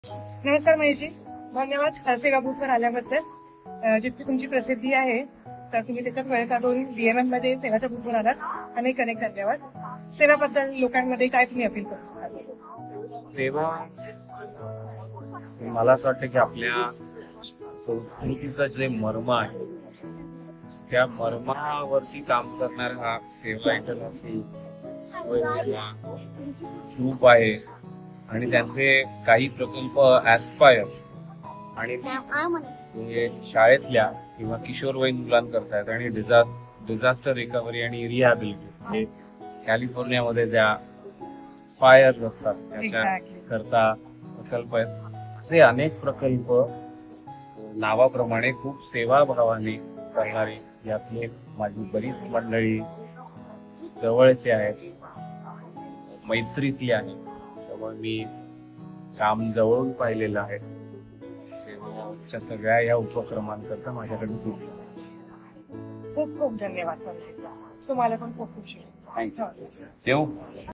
Mahesh Kale Praises Sewa International | BMM Convention 2024
We are honored to have the support of Mahesh Kale, the winner of the 63rd National Film Award of India for the Best Playback Singer. Based in the San Francisco Bay Area, he is also a strong admirer of Sewa International. Kale's recent interview during the Brihan Maharashtra Mandal Convention 2024 in San Jose, sheds light on Sewa's ethos and impact. His appreciation for our initiatives like ASPIRE, LEAD, and Disaster Management is heartwarming.

As a musician, Kale understands the power of sacrifice, service, and giving back to the community. His words about Sewa's work reflect the spirit of our organization. We are grateful for his support and encouragement of Sewa's projects."